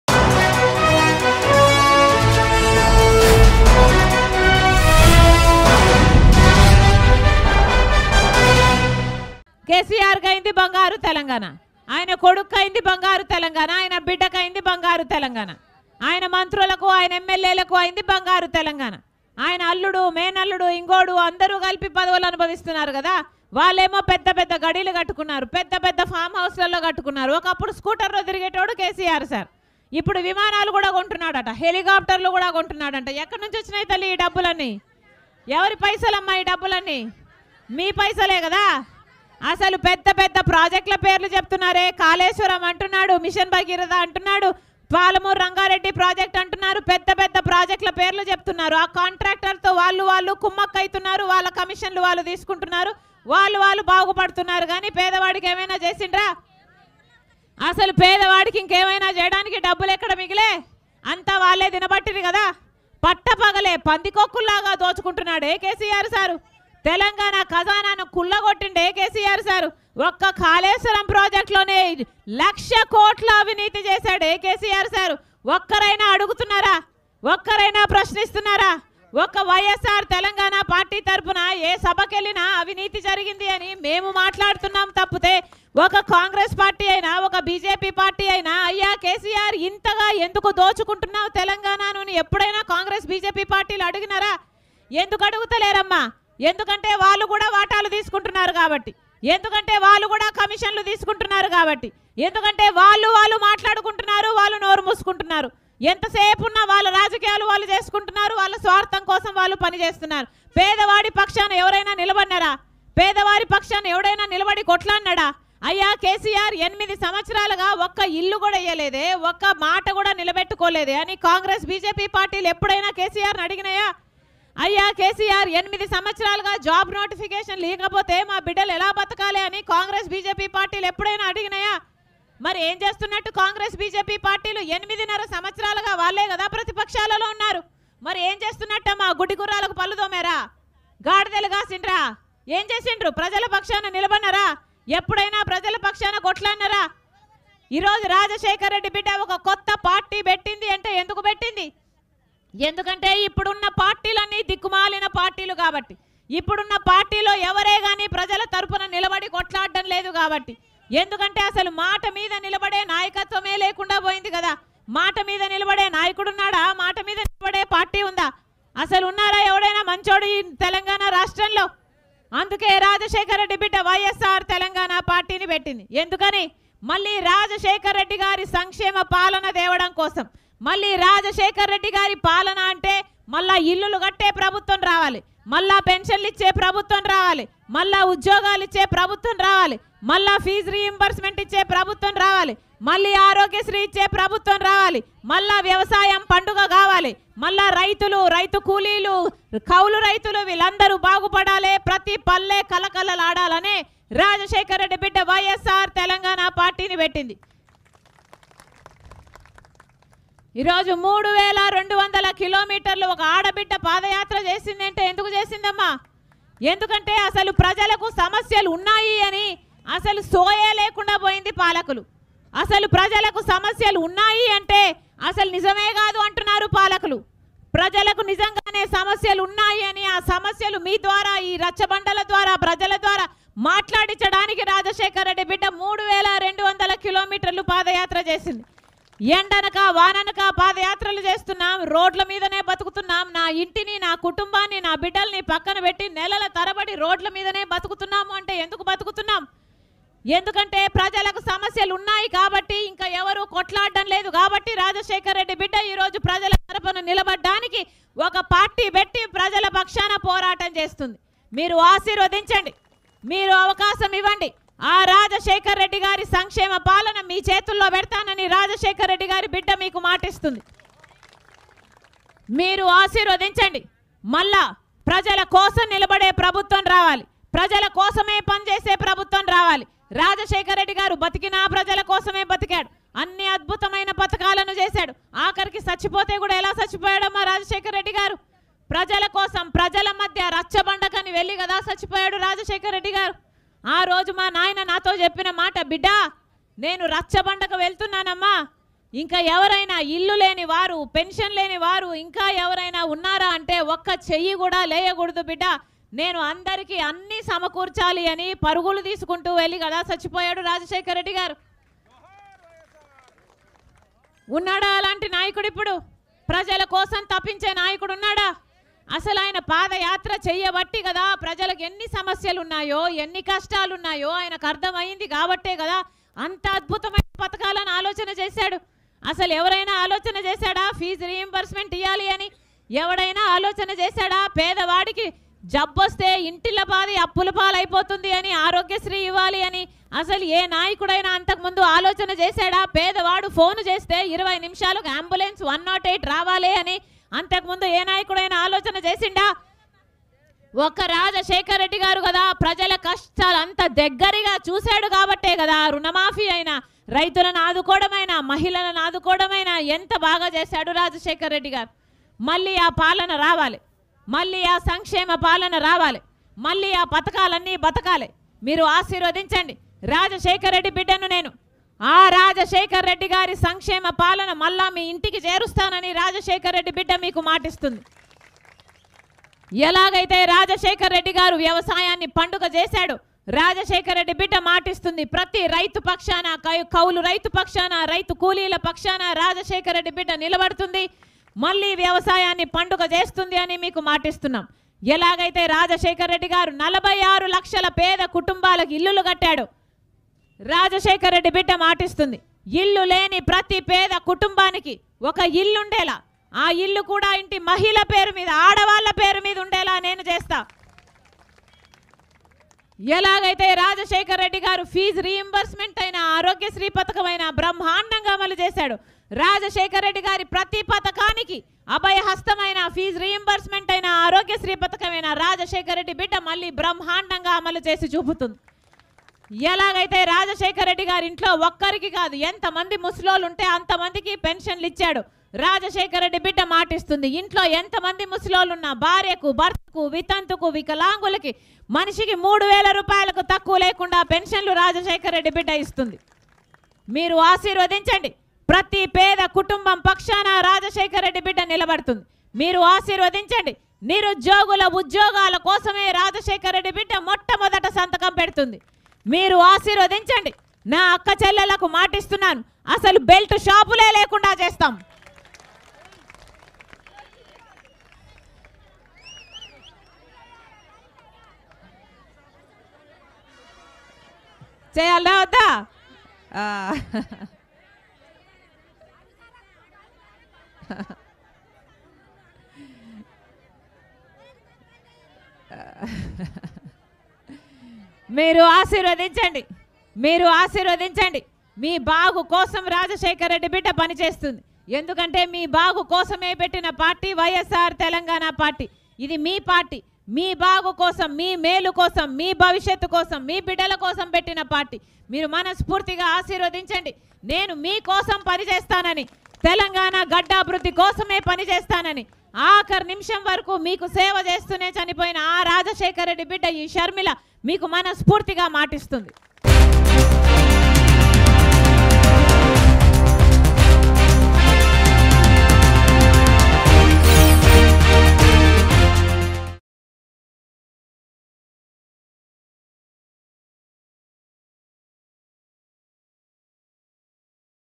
YS Sharmila Slams BJP & Congress Party | Karimnagar Public Meeting | YSRTPVsTRS | Mango News
YS Sharmila Slams BJP & Congress Party at Karimnagar Public Meeting.